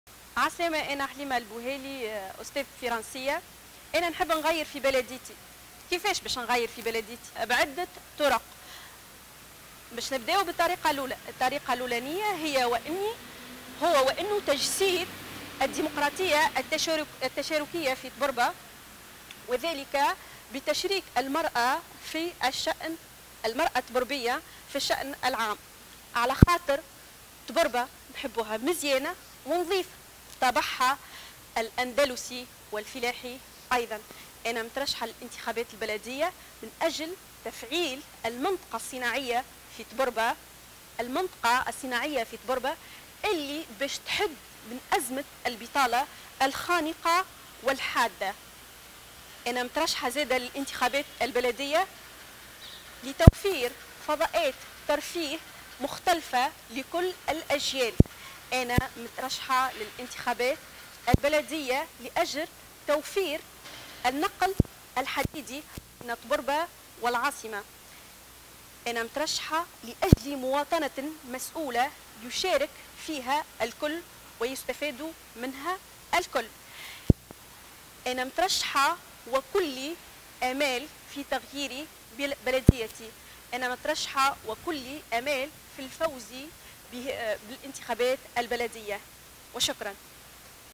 حليمة البوهالي، مرشحة للإنتخابات البلدية تونس
تقدم حليمة البوهالي، مرشحة للإنتخابات البلدية عن طبربة ولاية منوبة، تونس، مشروع قائمتها في إطار